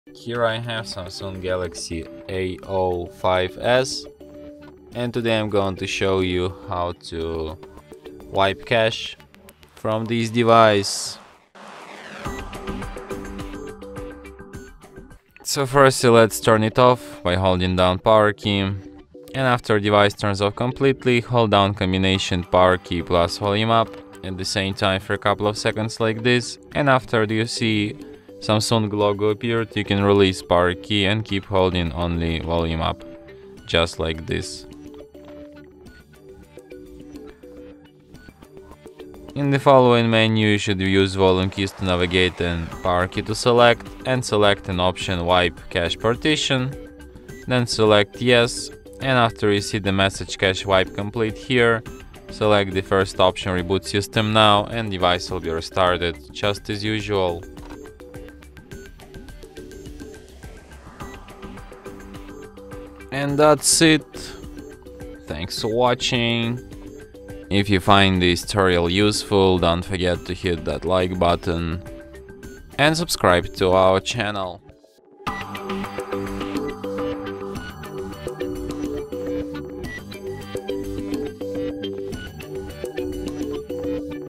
How to Wipe Cache in Samsung galaxy A05s – Remove Cache Partition
Hi there! Give your Samsung Galaxy A05s a fresh start by wiping the cache partition. This process is akin to clearing out cobwebs, ensuring your device runs smoothly. By removing temporary files and data clutter, you can experience improved performance and a more responsive phone. It's like a spring cleaning for your smartphone, helping you maintain the optimal functionality of your Galaxy A05s.

How to wipe cache partition in Samsung galaxy A05s?
How to reset cache files in Samsung galaxy A05s?
How to wipe cache in Samsung galaxy A05s?